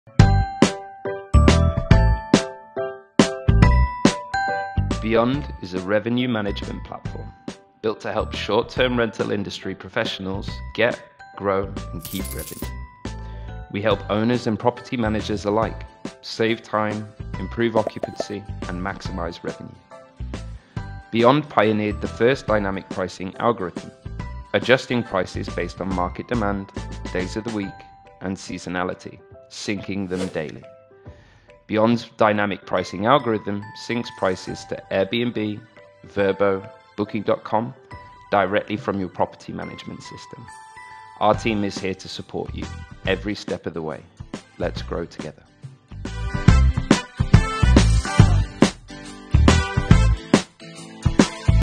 Welcome to Beyond
Beyond is a revenue management solution for short-term rental managers and owners to get, grow, and keep revenue.

Beyond connects seamlessly to your favorite booking platform. With more than 340,000+ listings in more than 7,500 cities worldwide, we're happy to say that we are the #1 dynamic pricing solution on the market. Let's grow together.

Talk to an expert and see if Beyond is right for you